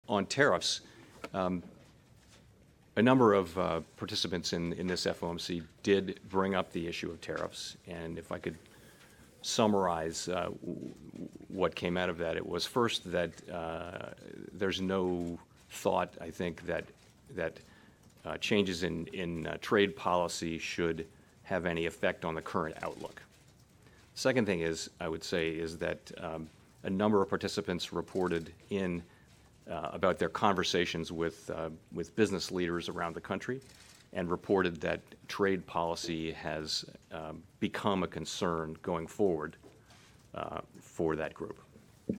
Powell Says FOMC Members Talked About Tariffs
Mar.21 -- Federal Reserve Chairman Jerome Powell speaks at a news conference in Washington about the decision by the Fed's policy-setting Federal Open Market Committee to raise the benchmark lending rate a quarter-point.